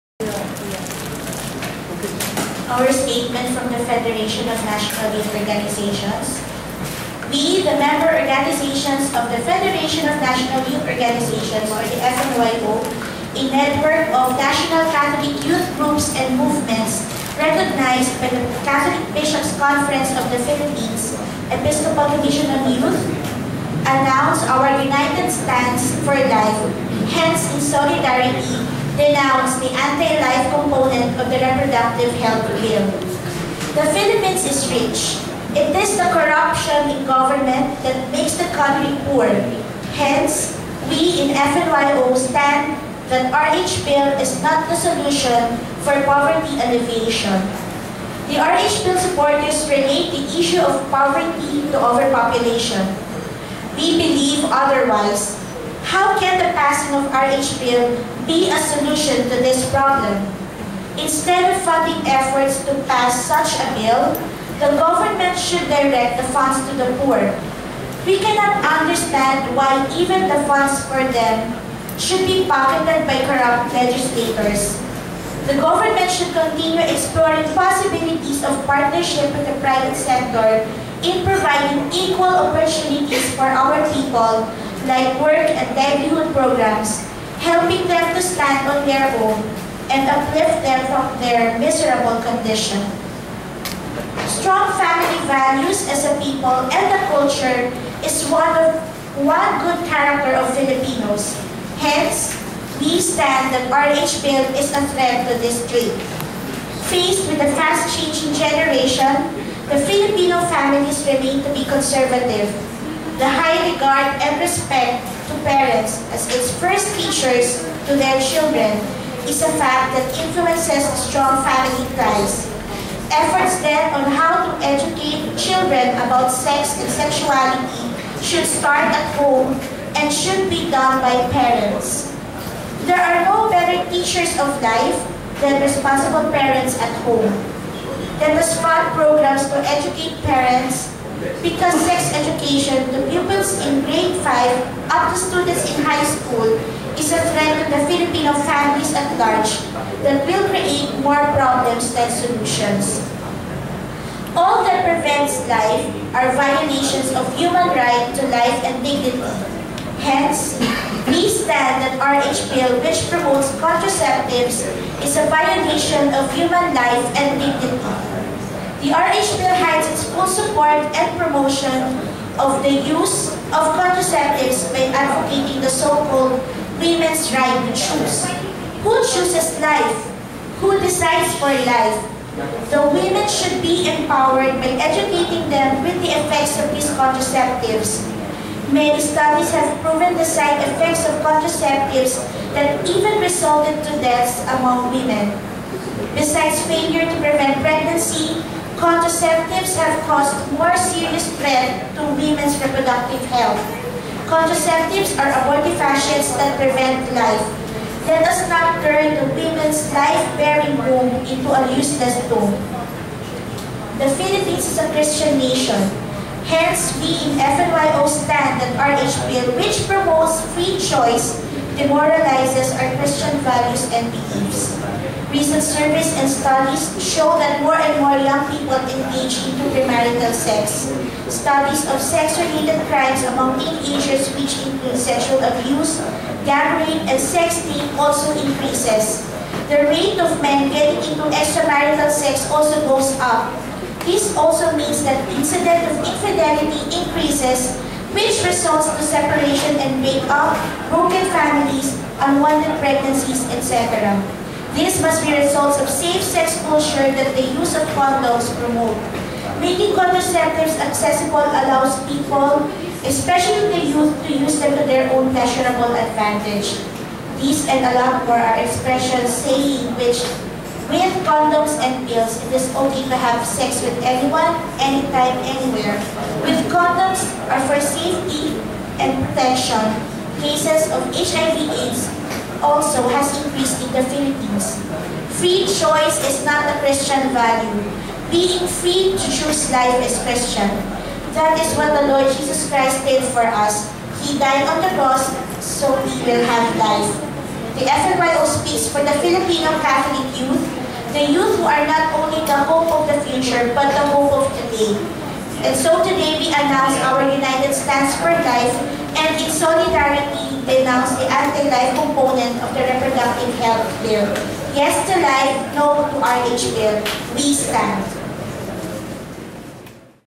Youth Alliance Against RH Bill - FNYO statement May 7, 2012
Youth Alliance Against RH Bill -  Federation of National Youth Organization statement May 7, 2012

MANILA, May 9, 2012--An alliance of youth organizations declared its all-out support for pro-life lawmakers, citing specifically the Nine Young Legislators (9YL), in a press conference in which representatives of various youth groups presented a manifesto proclaiming their position against the Reproductive Health (RH) bill.

"We would like to support the Nine Young Legislators who are basically the vanguards and who would [go against] the RH bill on behalf of the youth," said Eilleen Esteban, president of Youth Pinoy! when asked if the manifesto titled Our Voice, Our Vote was meant to send a message to those running in the 2013 elections.

"Pwede po nating sabihin na isa na ito sa mga pagkilos na gagawin namin para sa darating na eleksyon. Ang ginagawa po namin ngayon na pagsasama-sama, o [ang pagbubuo ng] coalition, is us forming a movement along with different Catholic and [non-Catholic] youth organizations whom we are inviting to join us with the same cause as we have to support our legislators who will be opposing the RH bill, and if needed, we will be there for them in 2013," Esteban said.

"This panel speaks for itself--the fact that the panelists can speak on behalf of numbers could mean that there is a substantial support for pro-life legislators in 2013," added Kiboy Tabada of UP for Life, a system-wide alliance of students, faculty, staff and alumni of the University of the Philippines.

The group 9YL, the members of which decided to band together late last year to express the need for government to allocate the billions proposed for the RH bill's implementation to more concrete solutions that will address the real needs of the people, is composed of Rep. Dakila Cua, Rachel Del Mar, LucyTorres-Gomez, Fatima Aliah Dimaporo, Karlo Nograles, Mariano Michael Velarde, Irwin Tieng, Gabriel Quisumbing, and Lord Allan Velasco.

Besides Esteban and Tabada, the other youth leaders who delivered statements proclaiming their respective groups' rejection of the RH bill were Lea Dasigan of the Federation of National Youth Organizations (FNYO), Peter Pardo of the National Capital Region Youth Ministry, Raymond Ibarrientos of CFC-Singles for Christ and Youth for Christ, and Allen Paolo Guballa of the Columbian Squires.

"We are for responsible parenthood and we, too, desire to see every Filipino family free from the burdens of poverty.  However, spending for a measure to fiddle with demographics is a gross misallocation of scarce resources," the manifesto stated.

"Addressing needs for quality education and opportunities for employment hits poverty at its core. Massive government spending for contraception and mandatory sex education in all schools enlivens the risk of aggravating the decline of our demographics, of eroding our values, and of reshaping our society after the designs of foreign intervention. There is no warrant against this. This is a gamble of our future."

"We are for population management and development and we, too, dream of progress. However, genuine progress can never be achieved until our government invests genuinely in our people.  Our population is an asset. Government must treat it as an asset," the manifesto, titled Our Voice, Our Vote, stated.

"Rural development, entrepreneurship, improved standards of education, expansion of opportunities for employment, intensified thrusts for scientific research and development, cultivation of our arts and culture -- these constitute genuine investments in our people. This is the way to secure our future."

"The youth, in defense of our welfare, can and will invest our support in legislators who know how to genuinely invest in us.

"In solidarity, we declare our opposition to the RH Bill. This is our voice. This is our vote," it concluded.